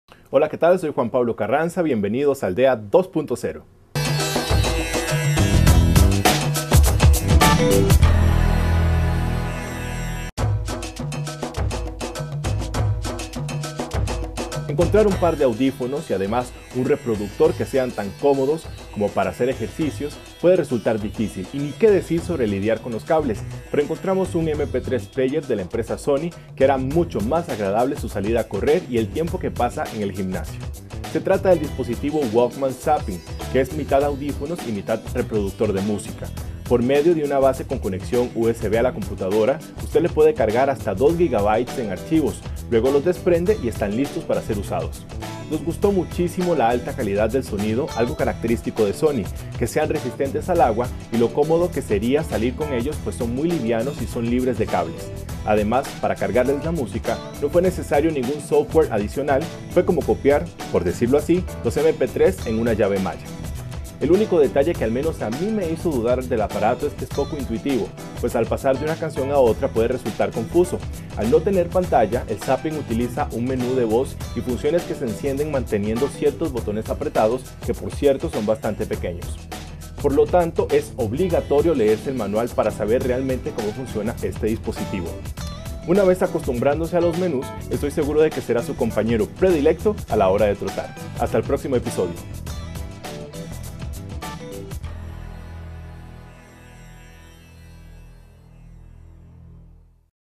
Walkman Zappin revisión
En Aldea 2.0 revisamos el reproductor de mp3 Walkman Zappin, de Sony, un aparato ideal para los que les gusta hacer deporte y disfrutar de la música al mismo tiempo.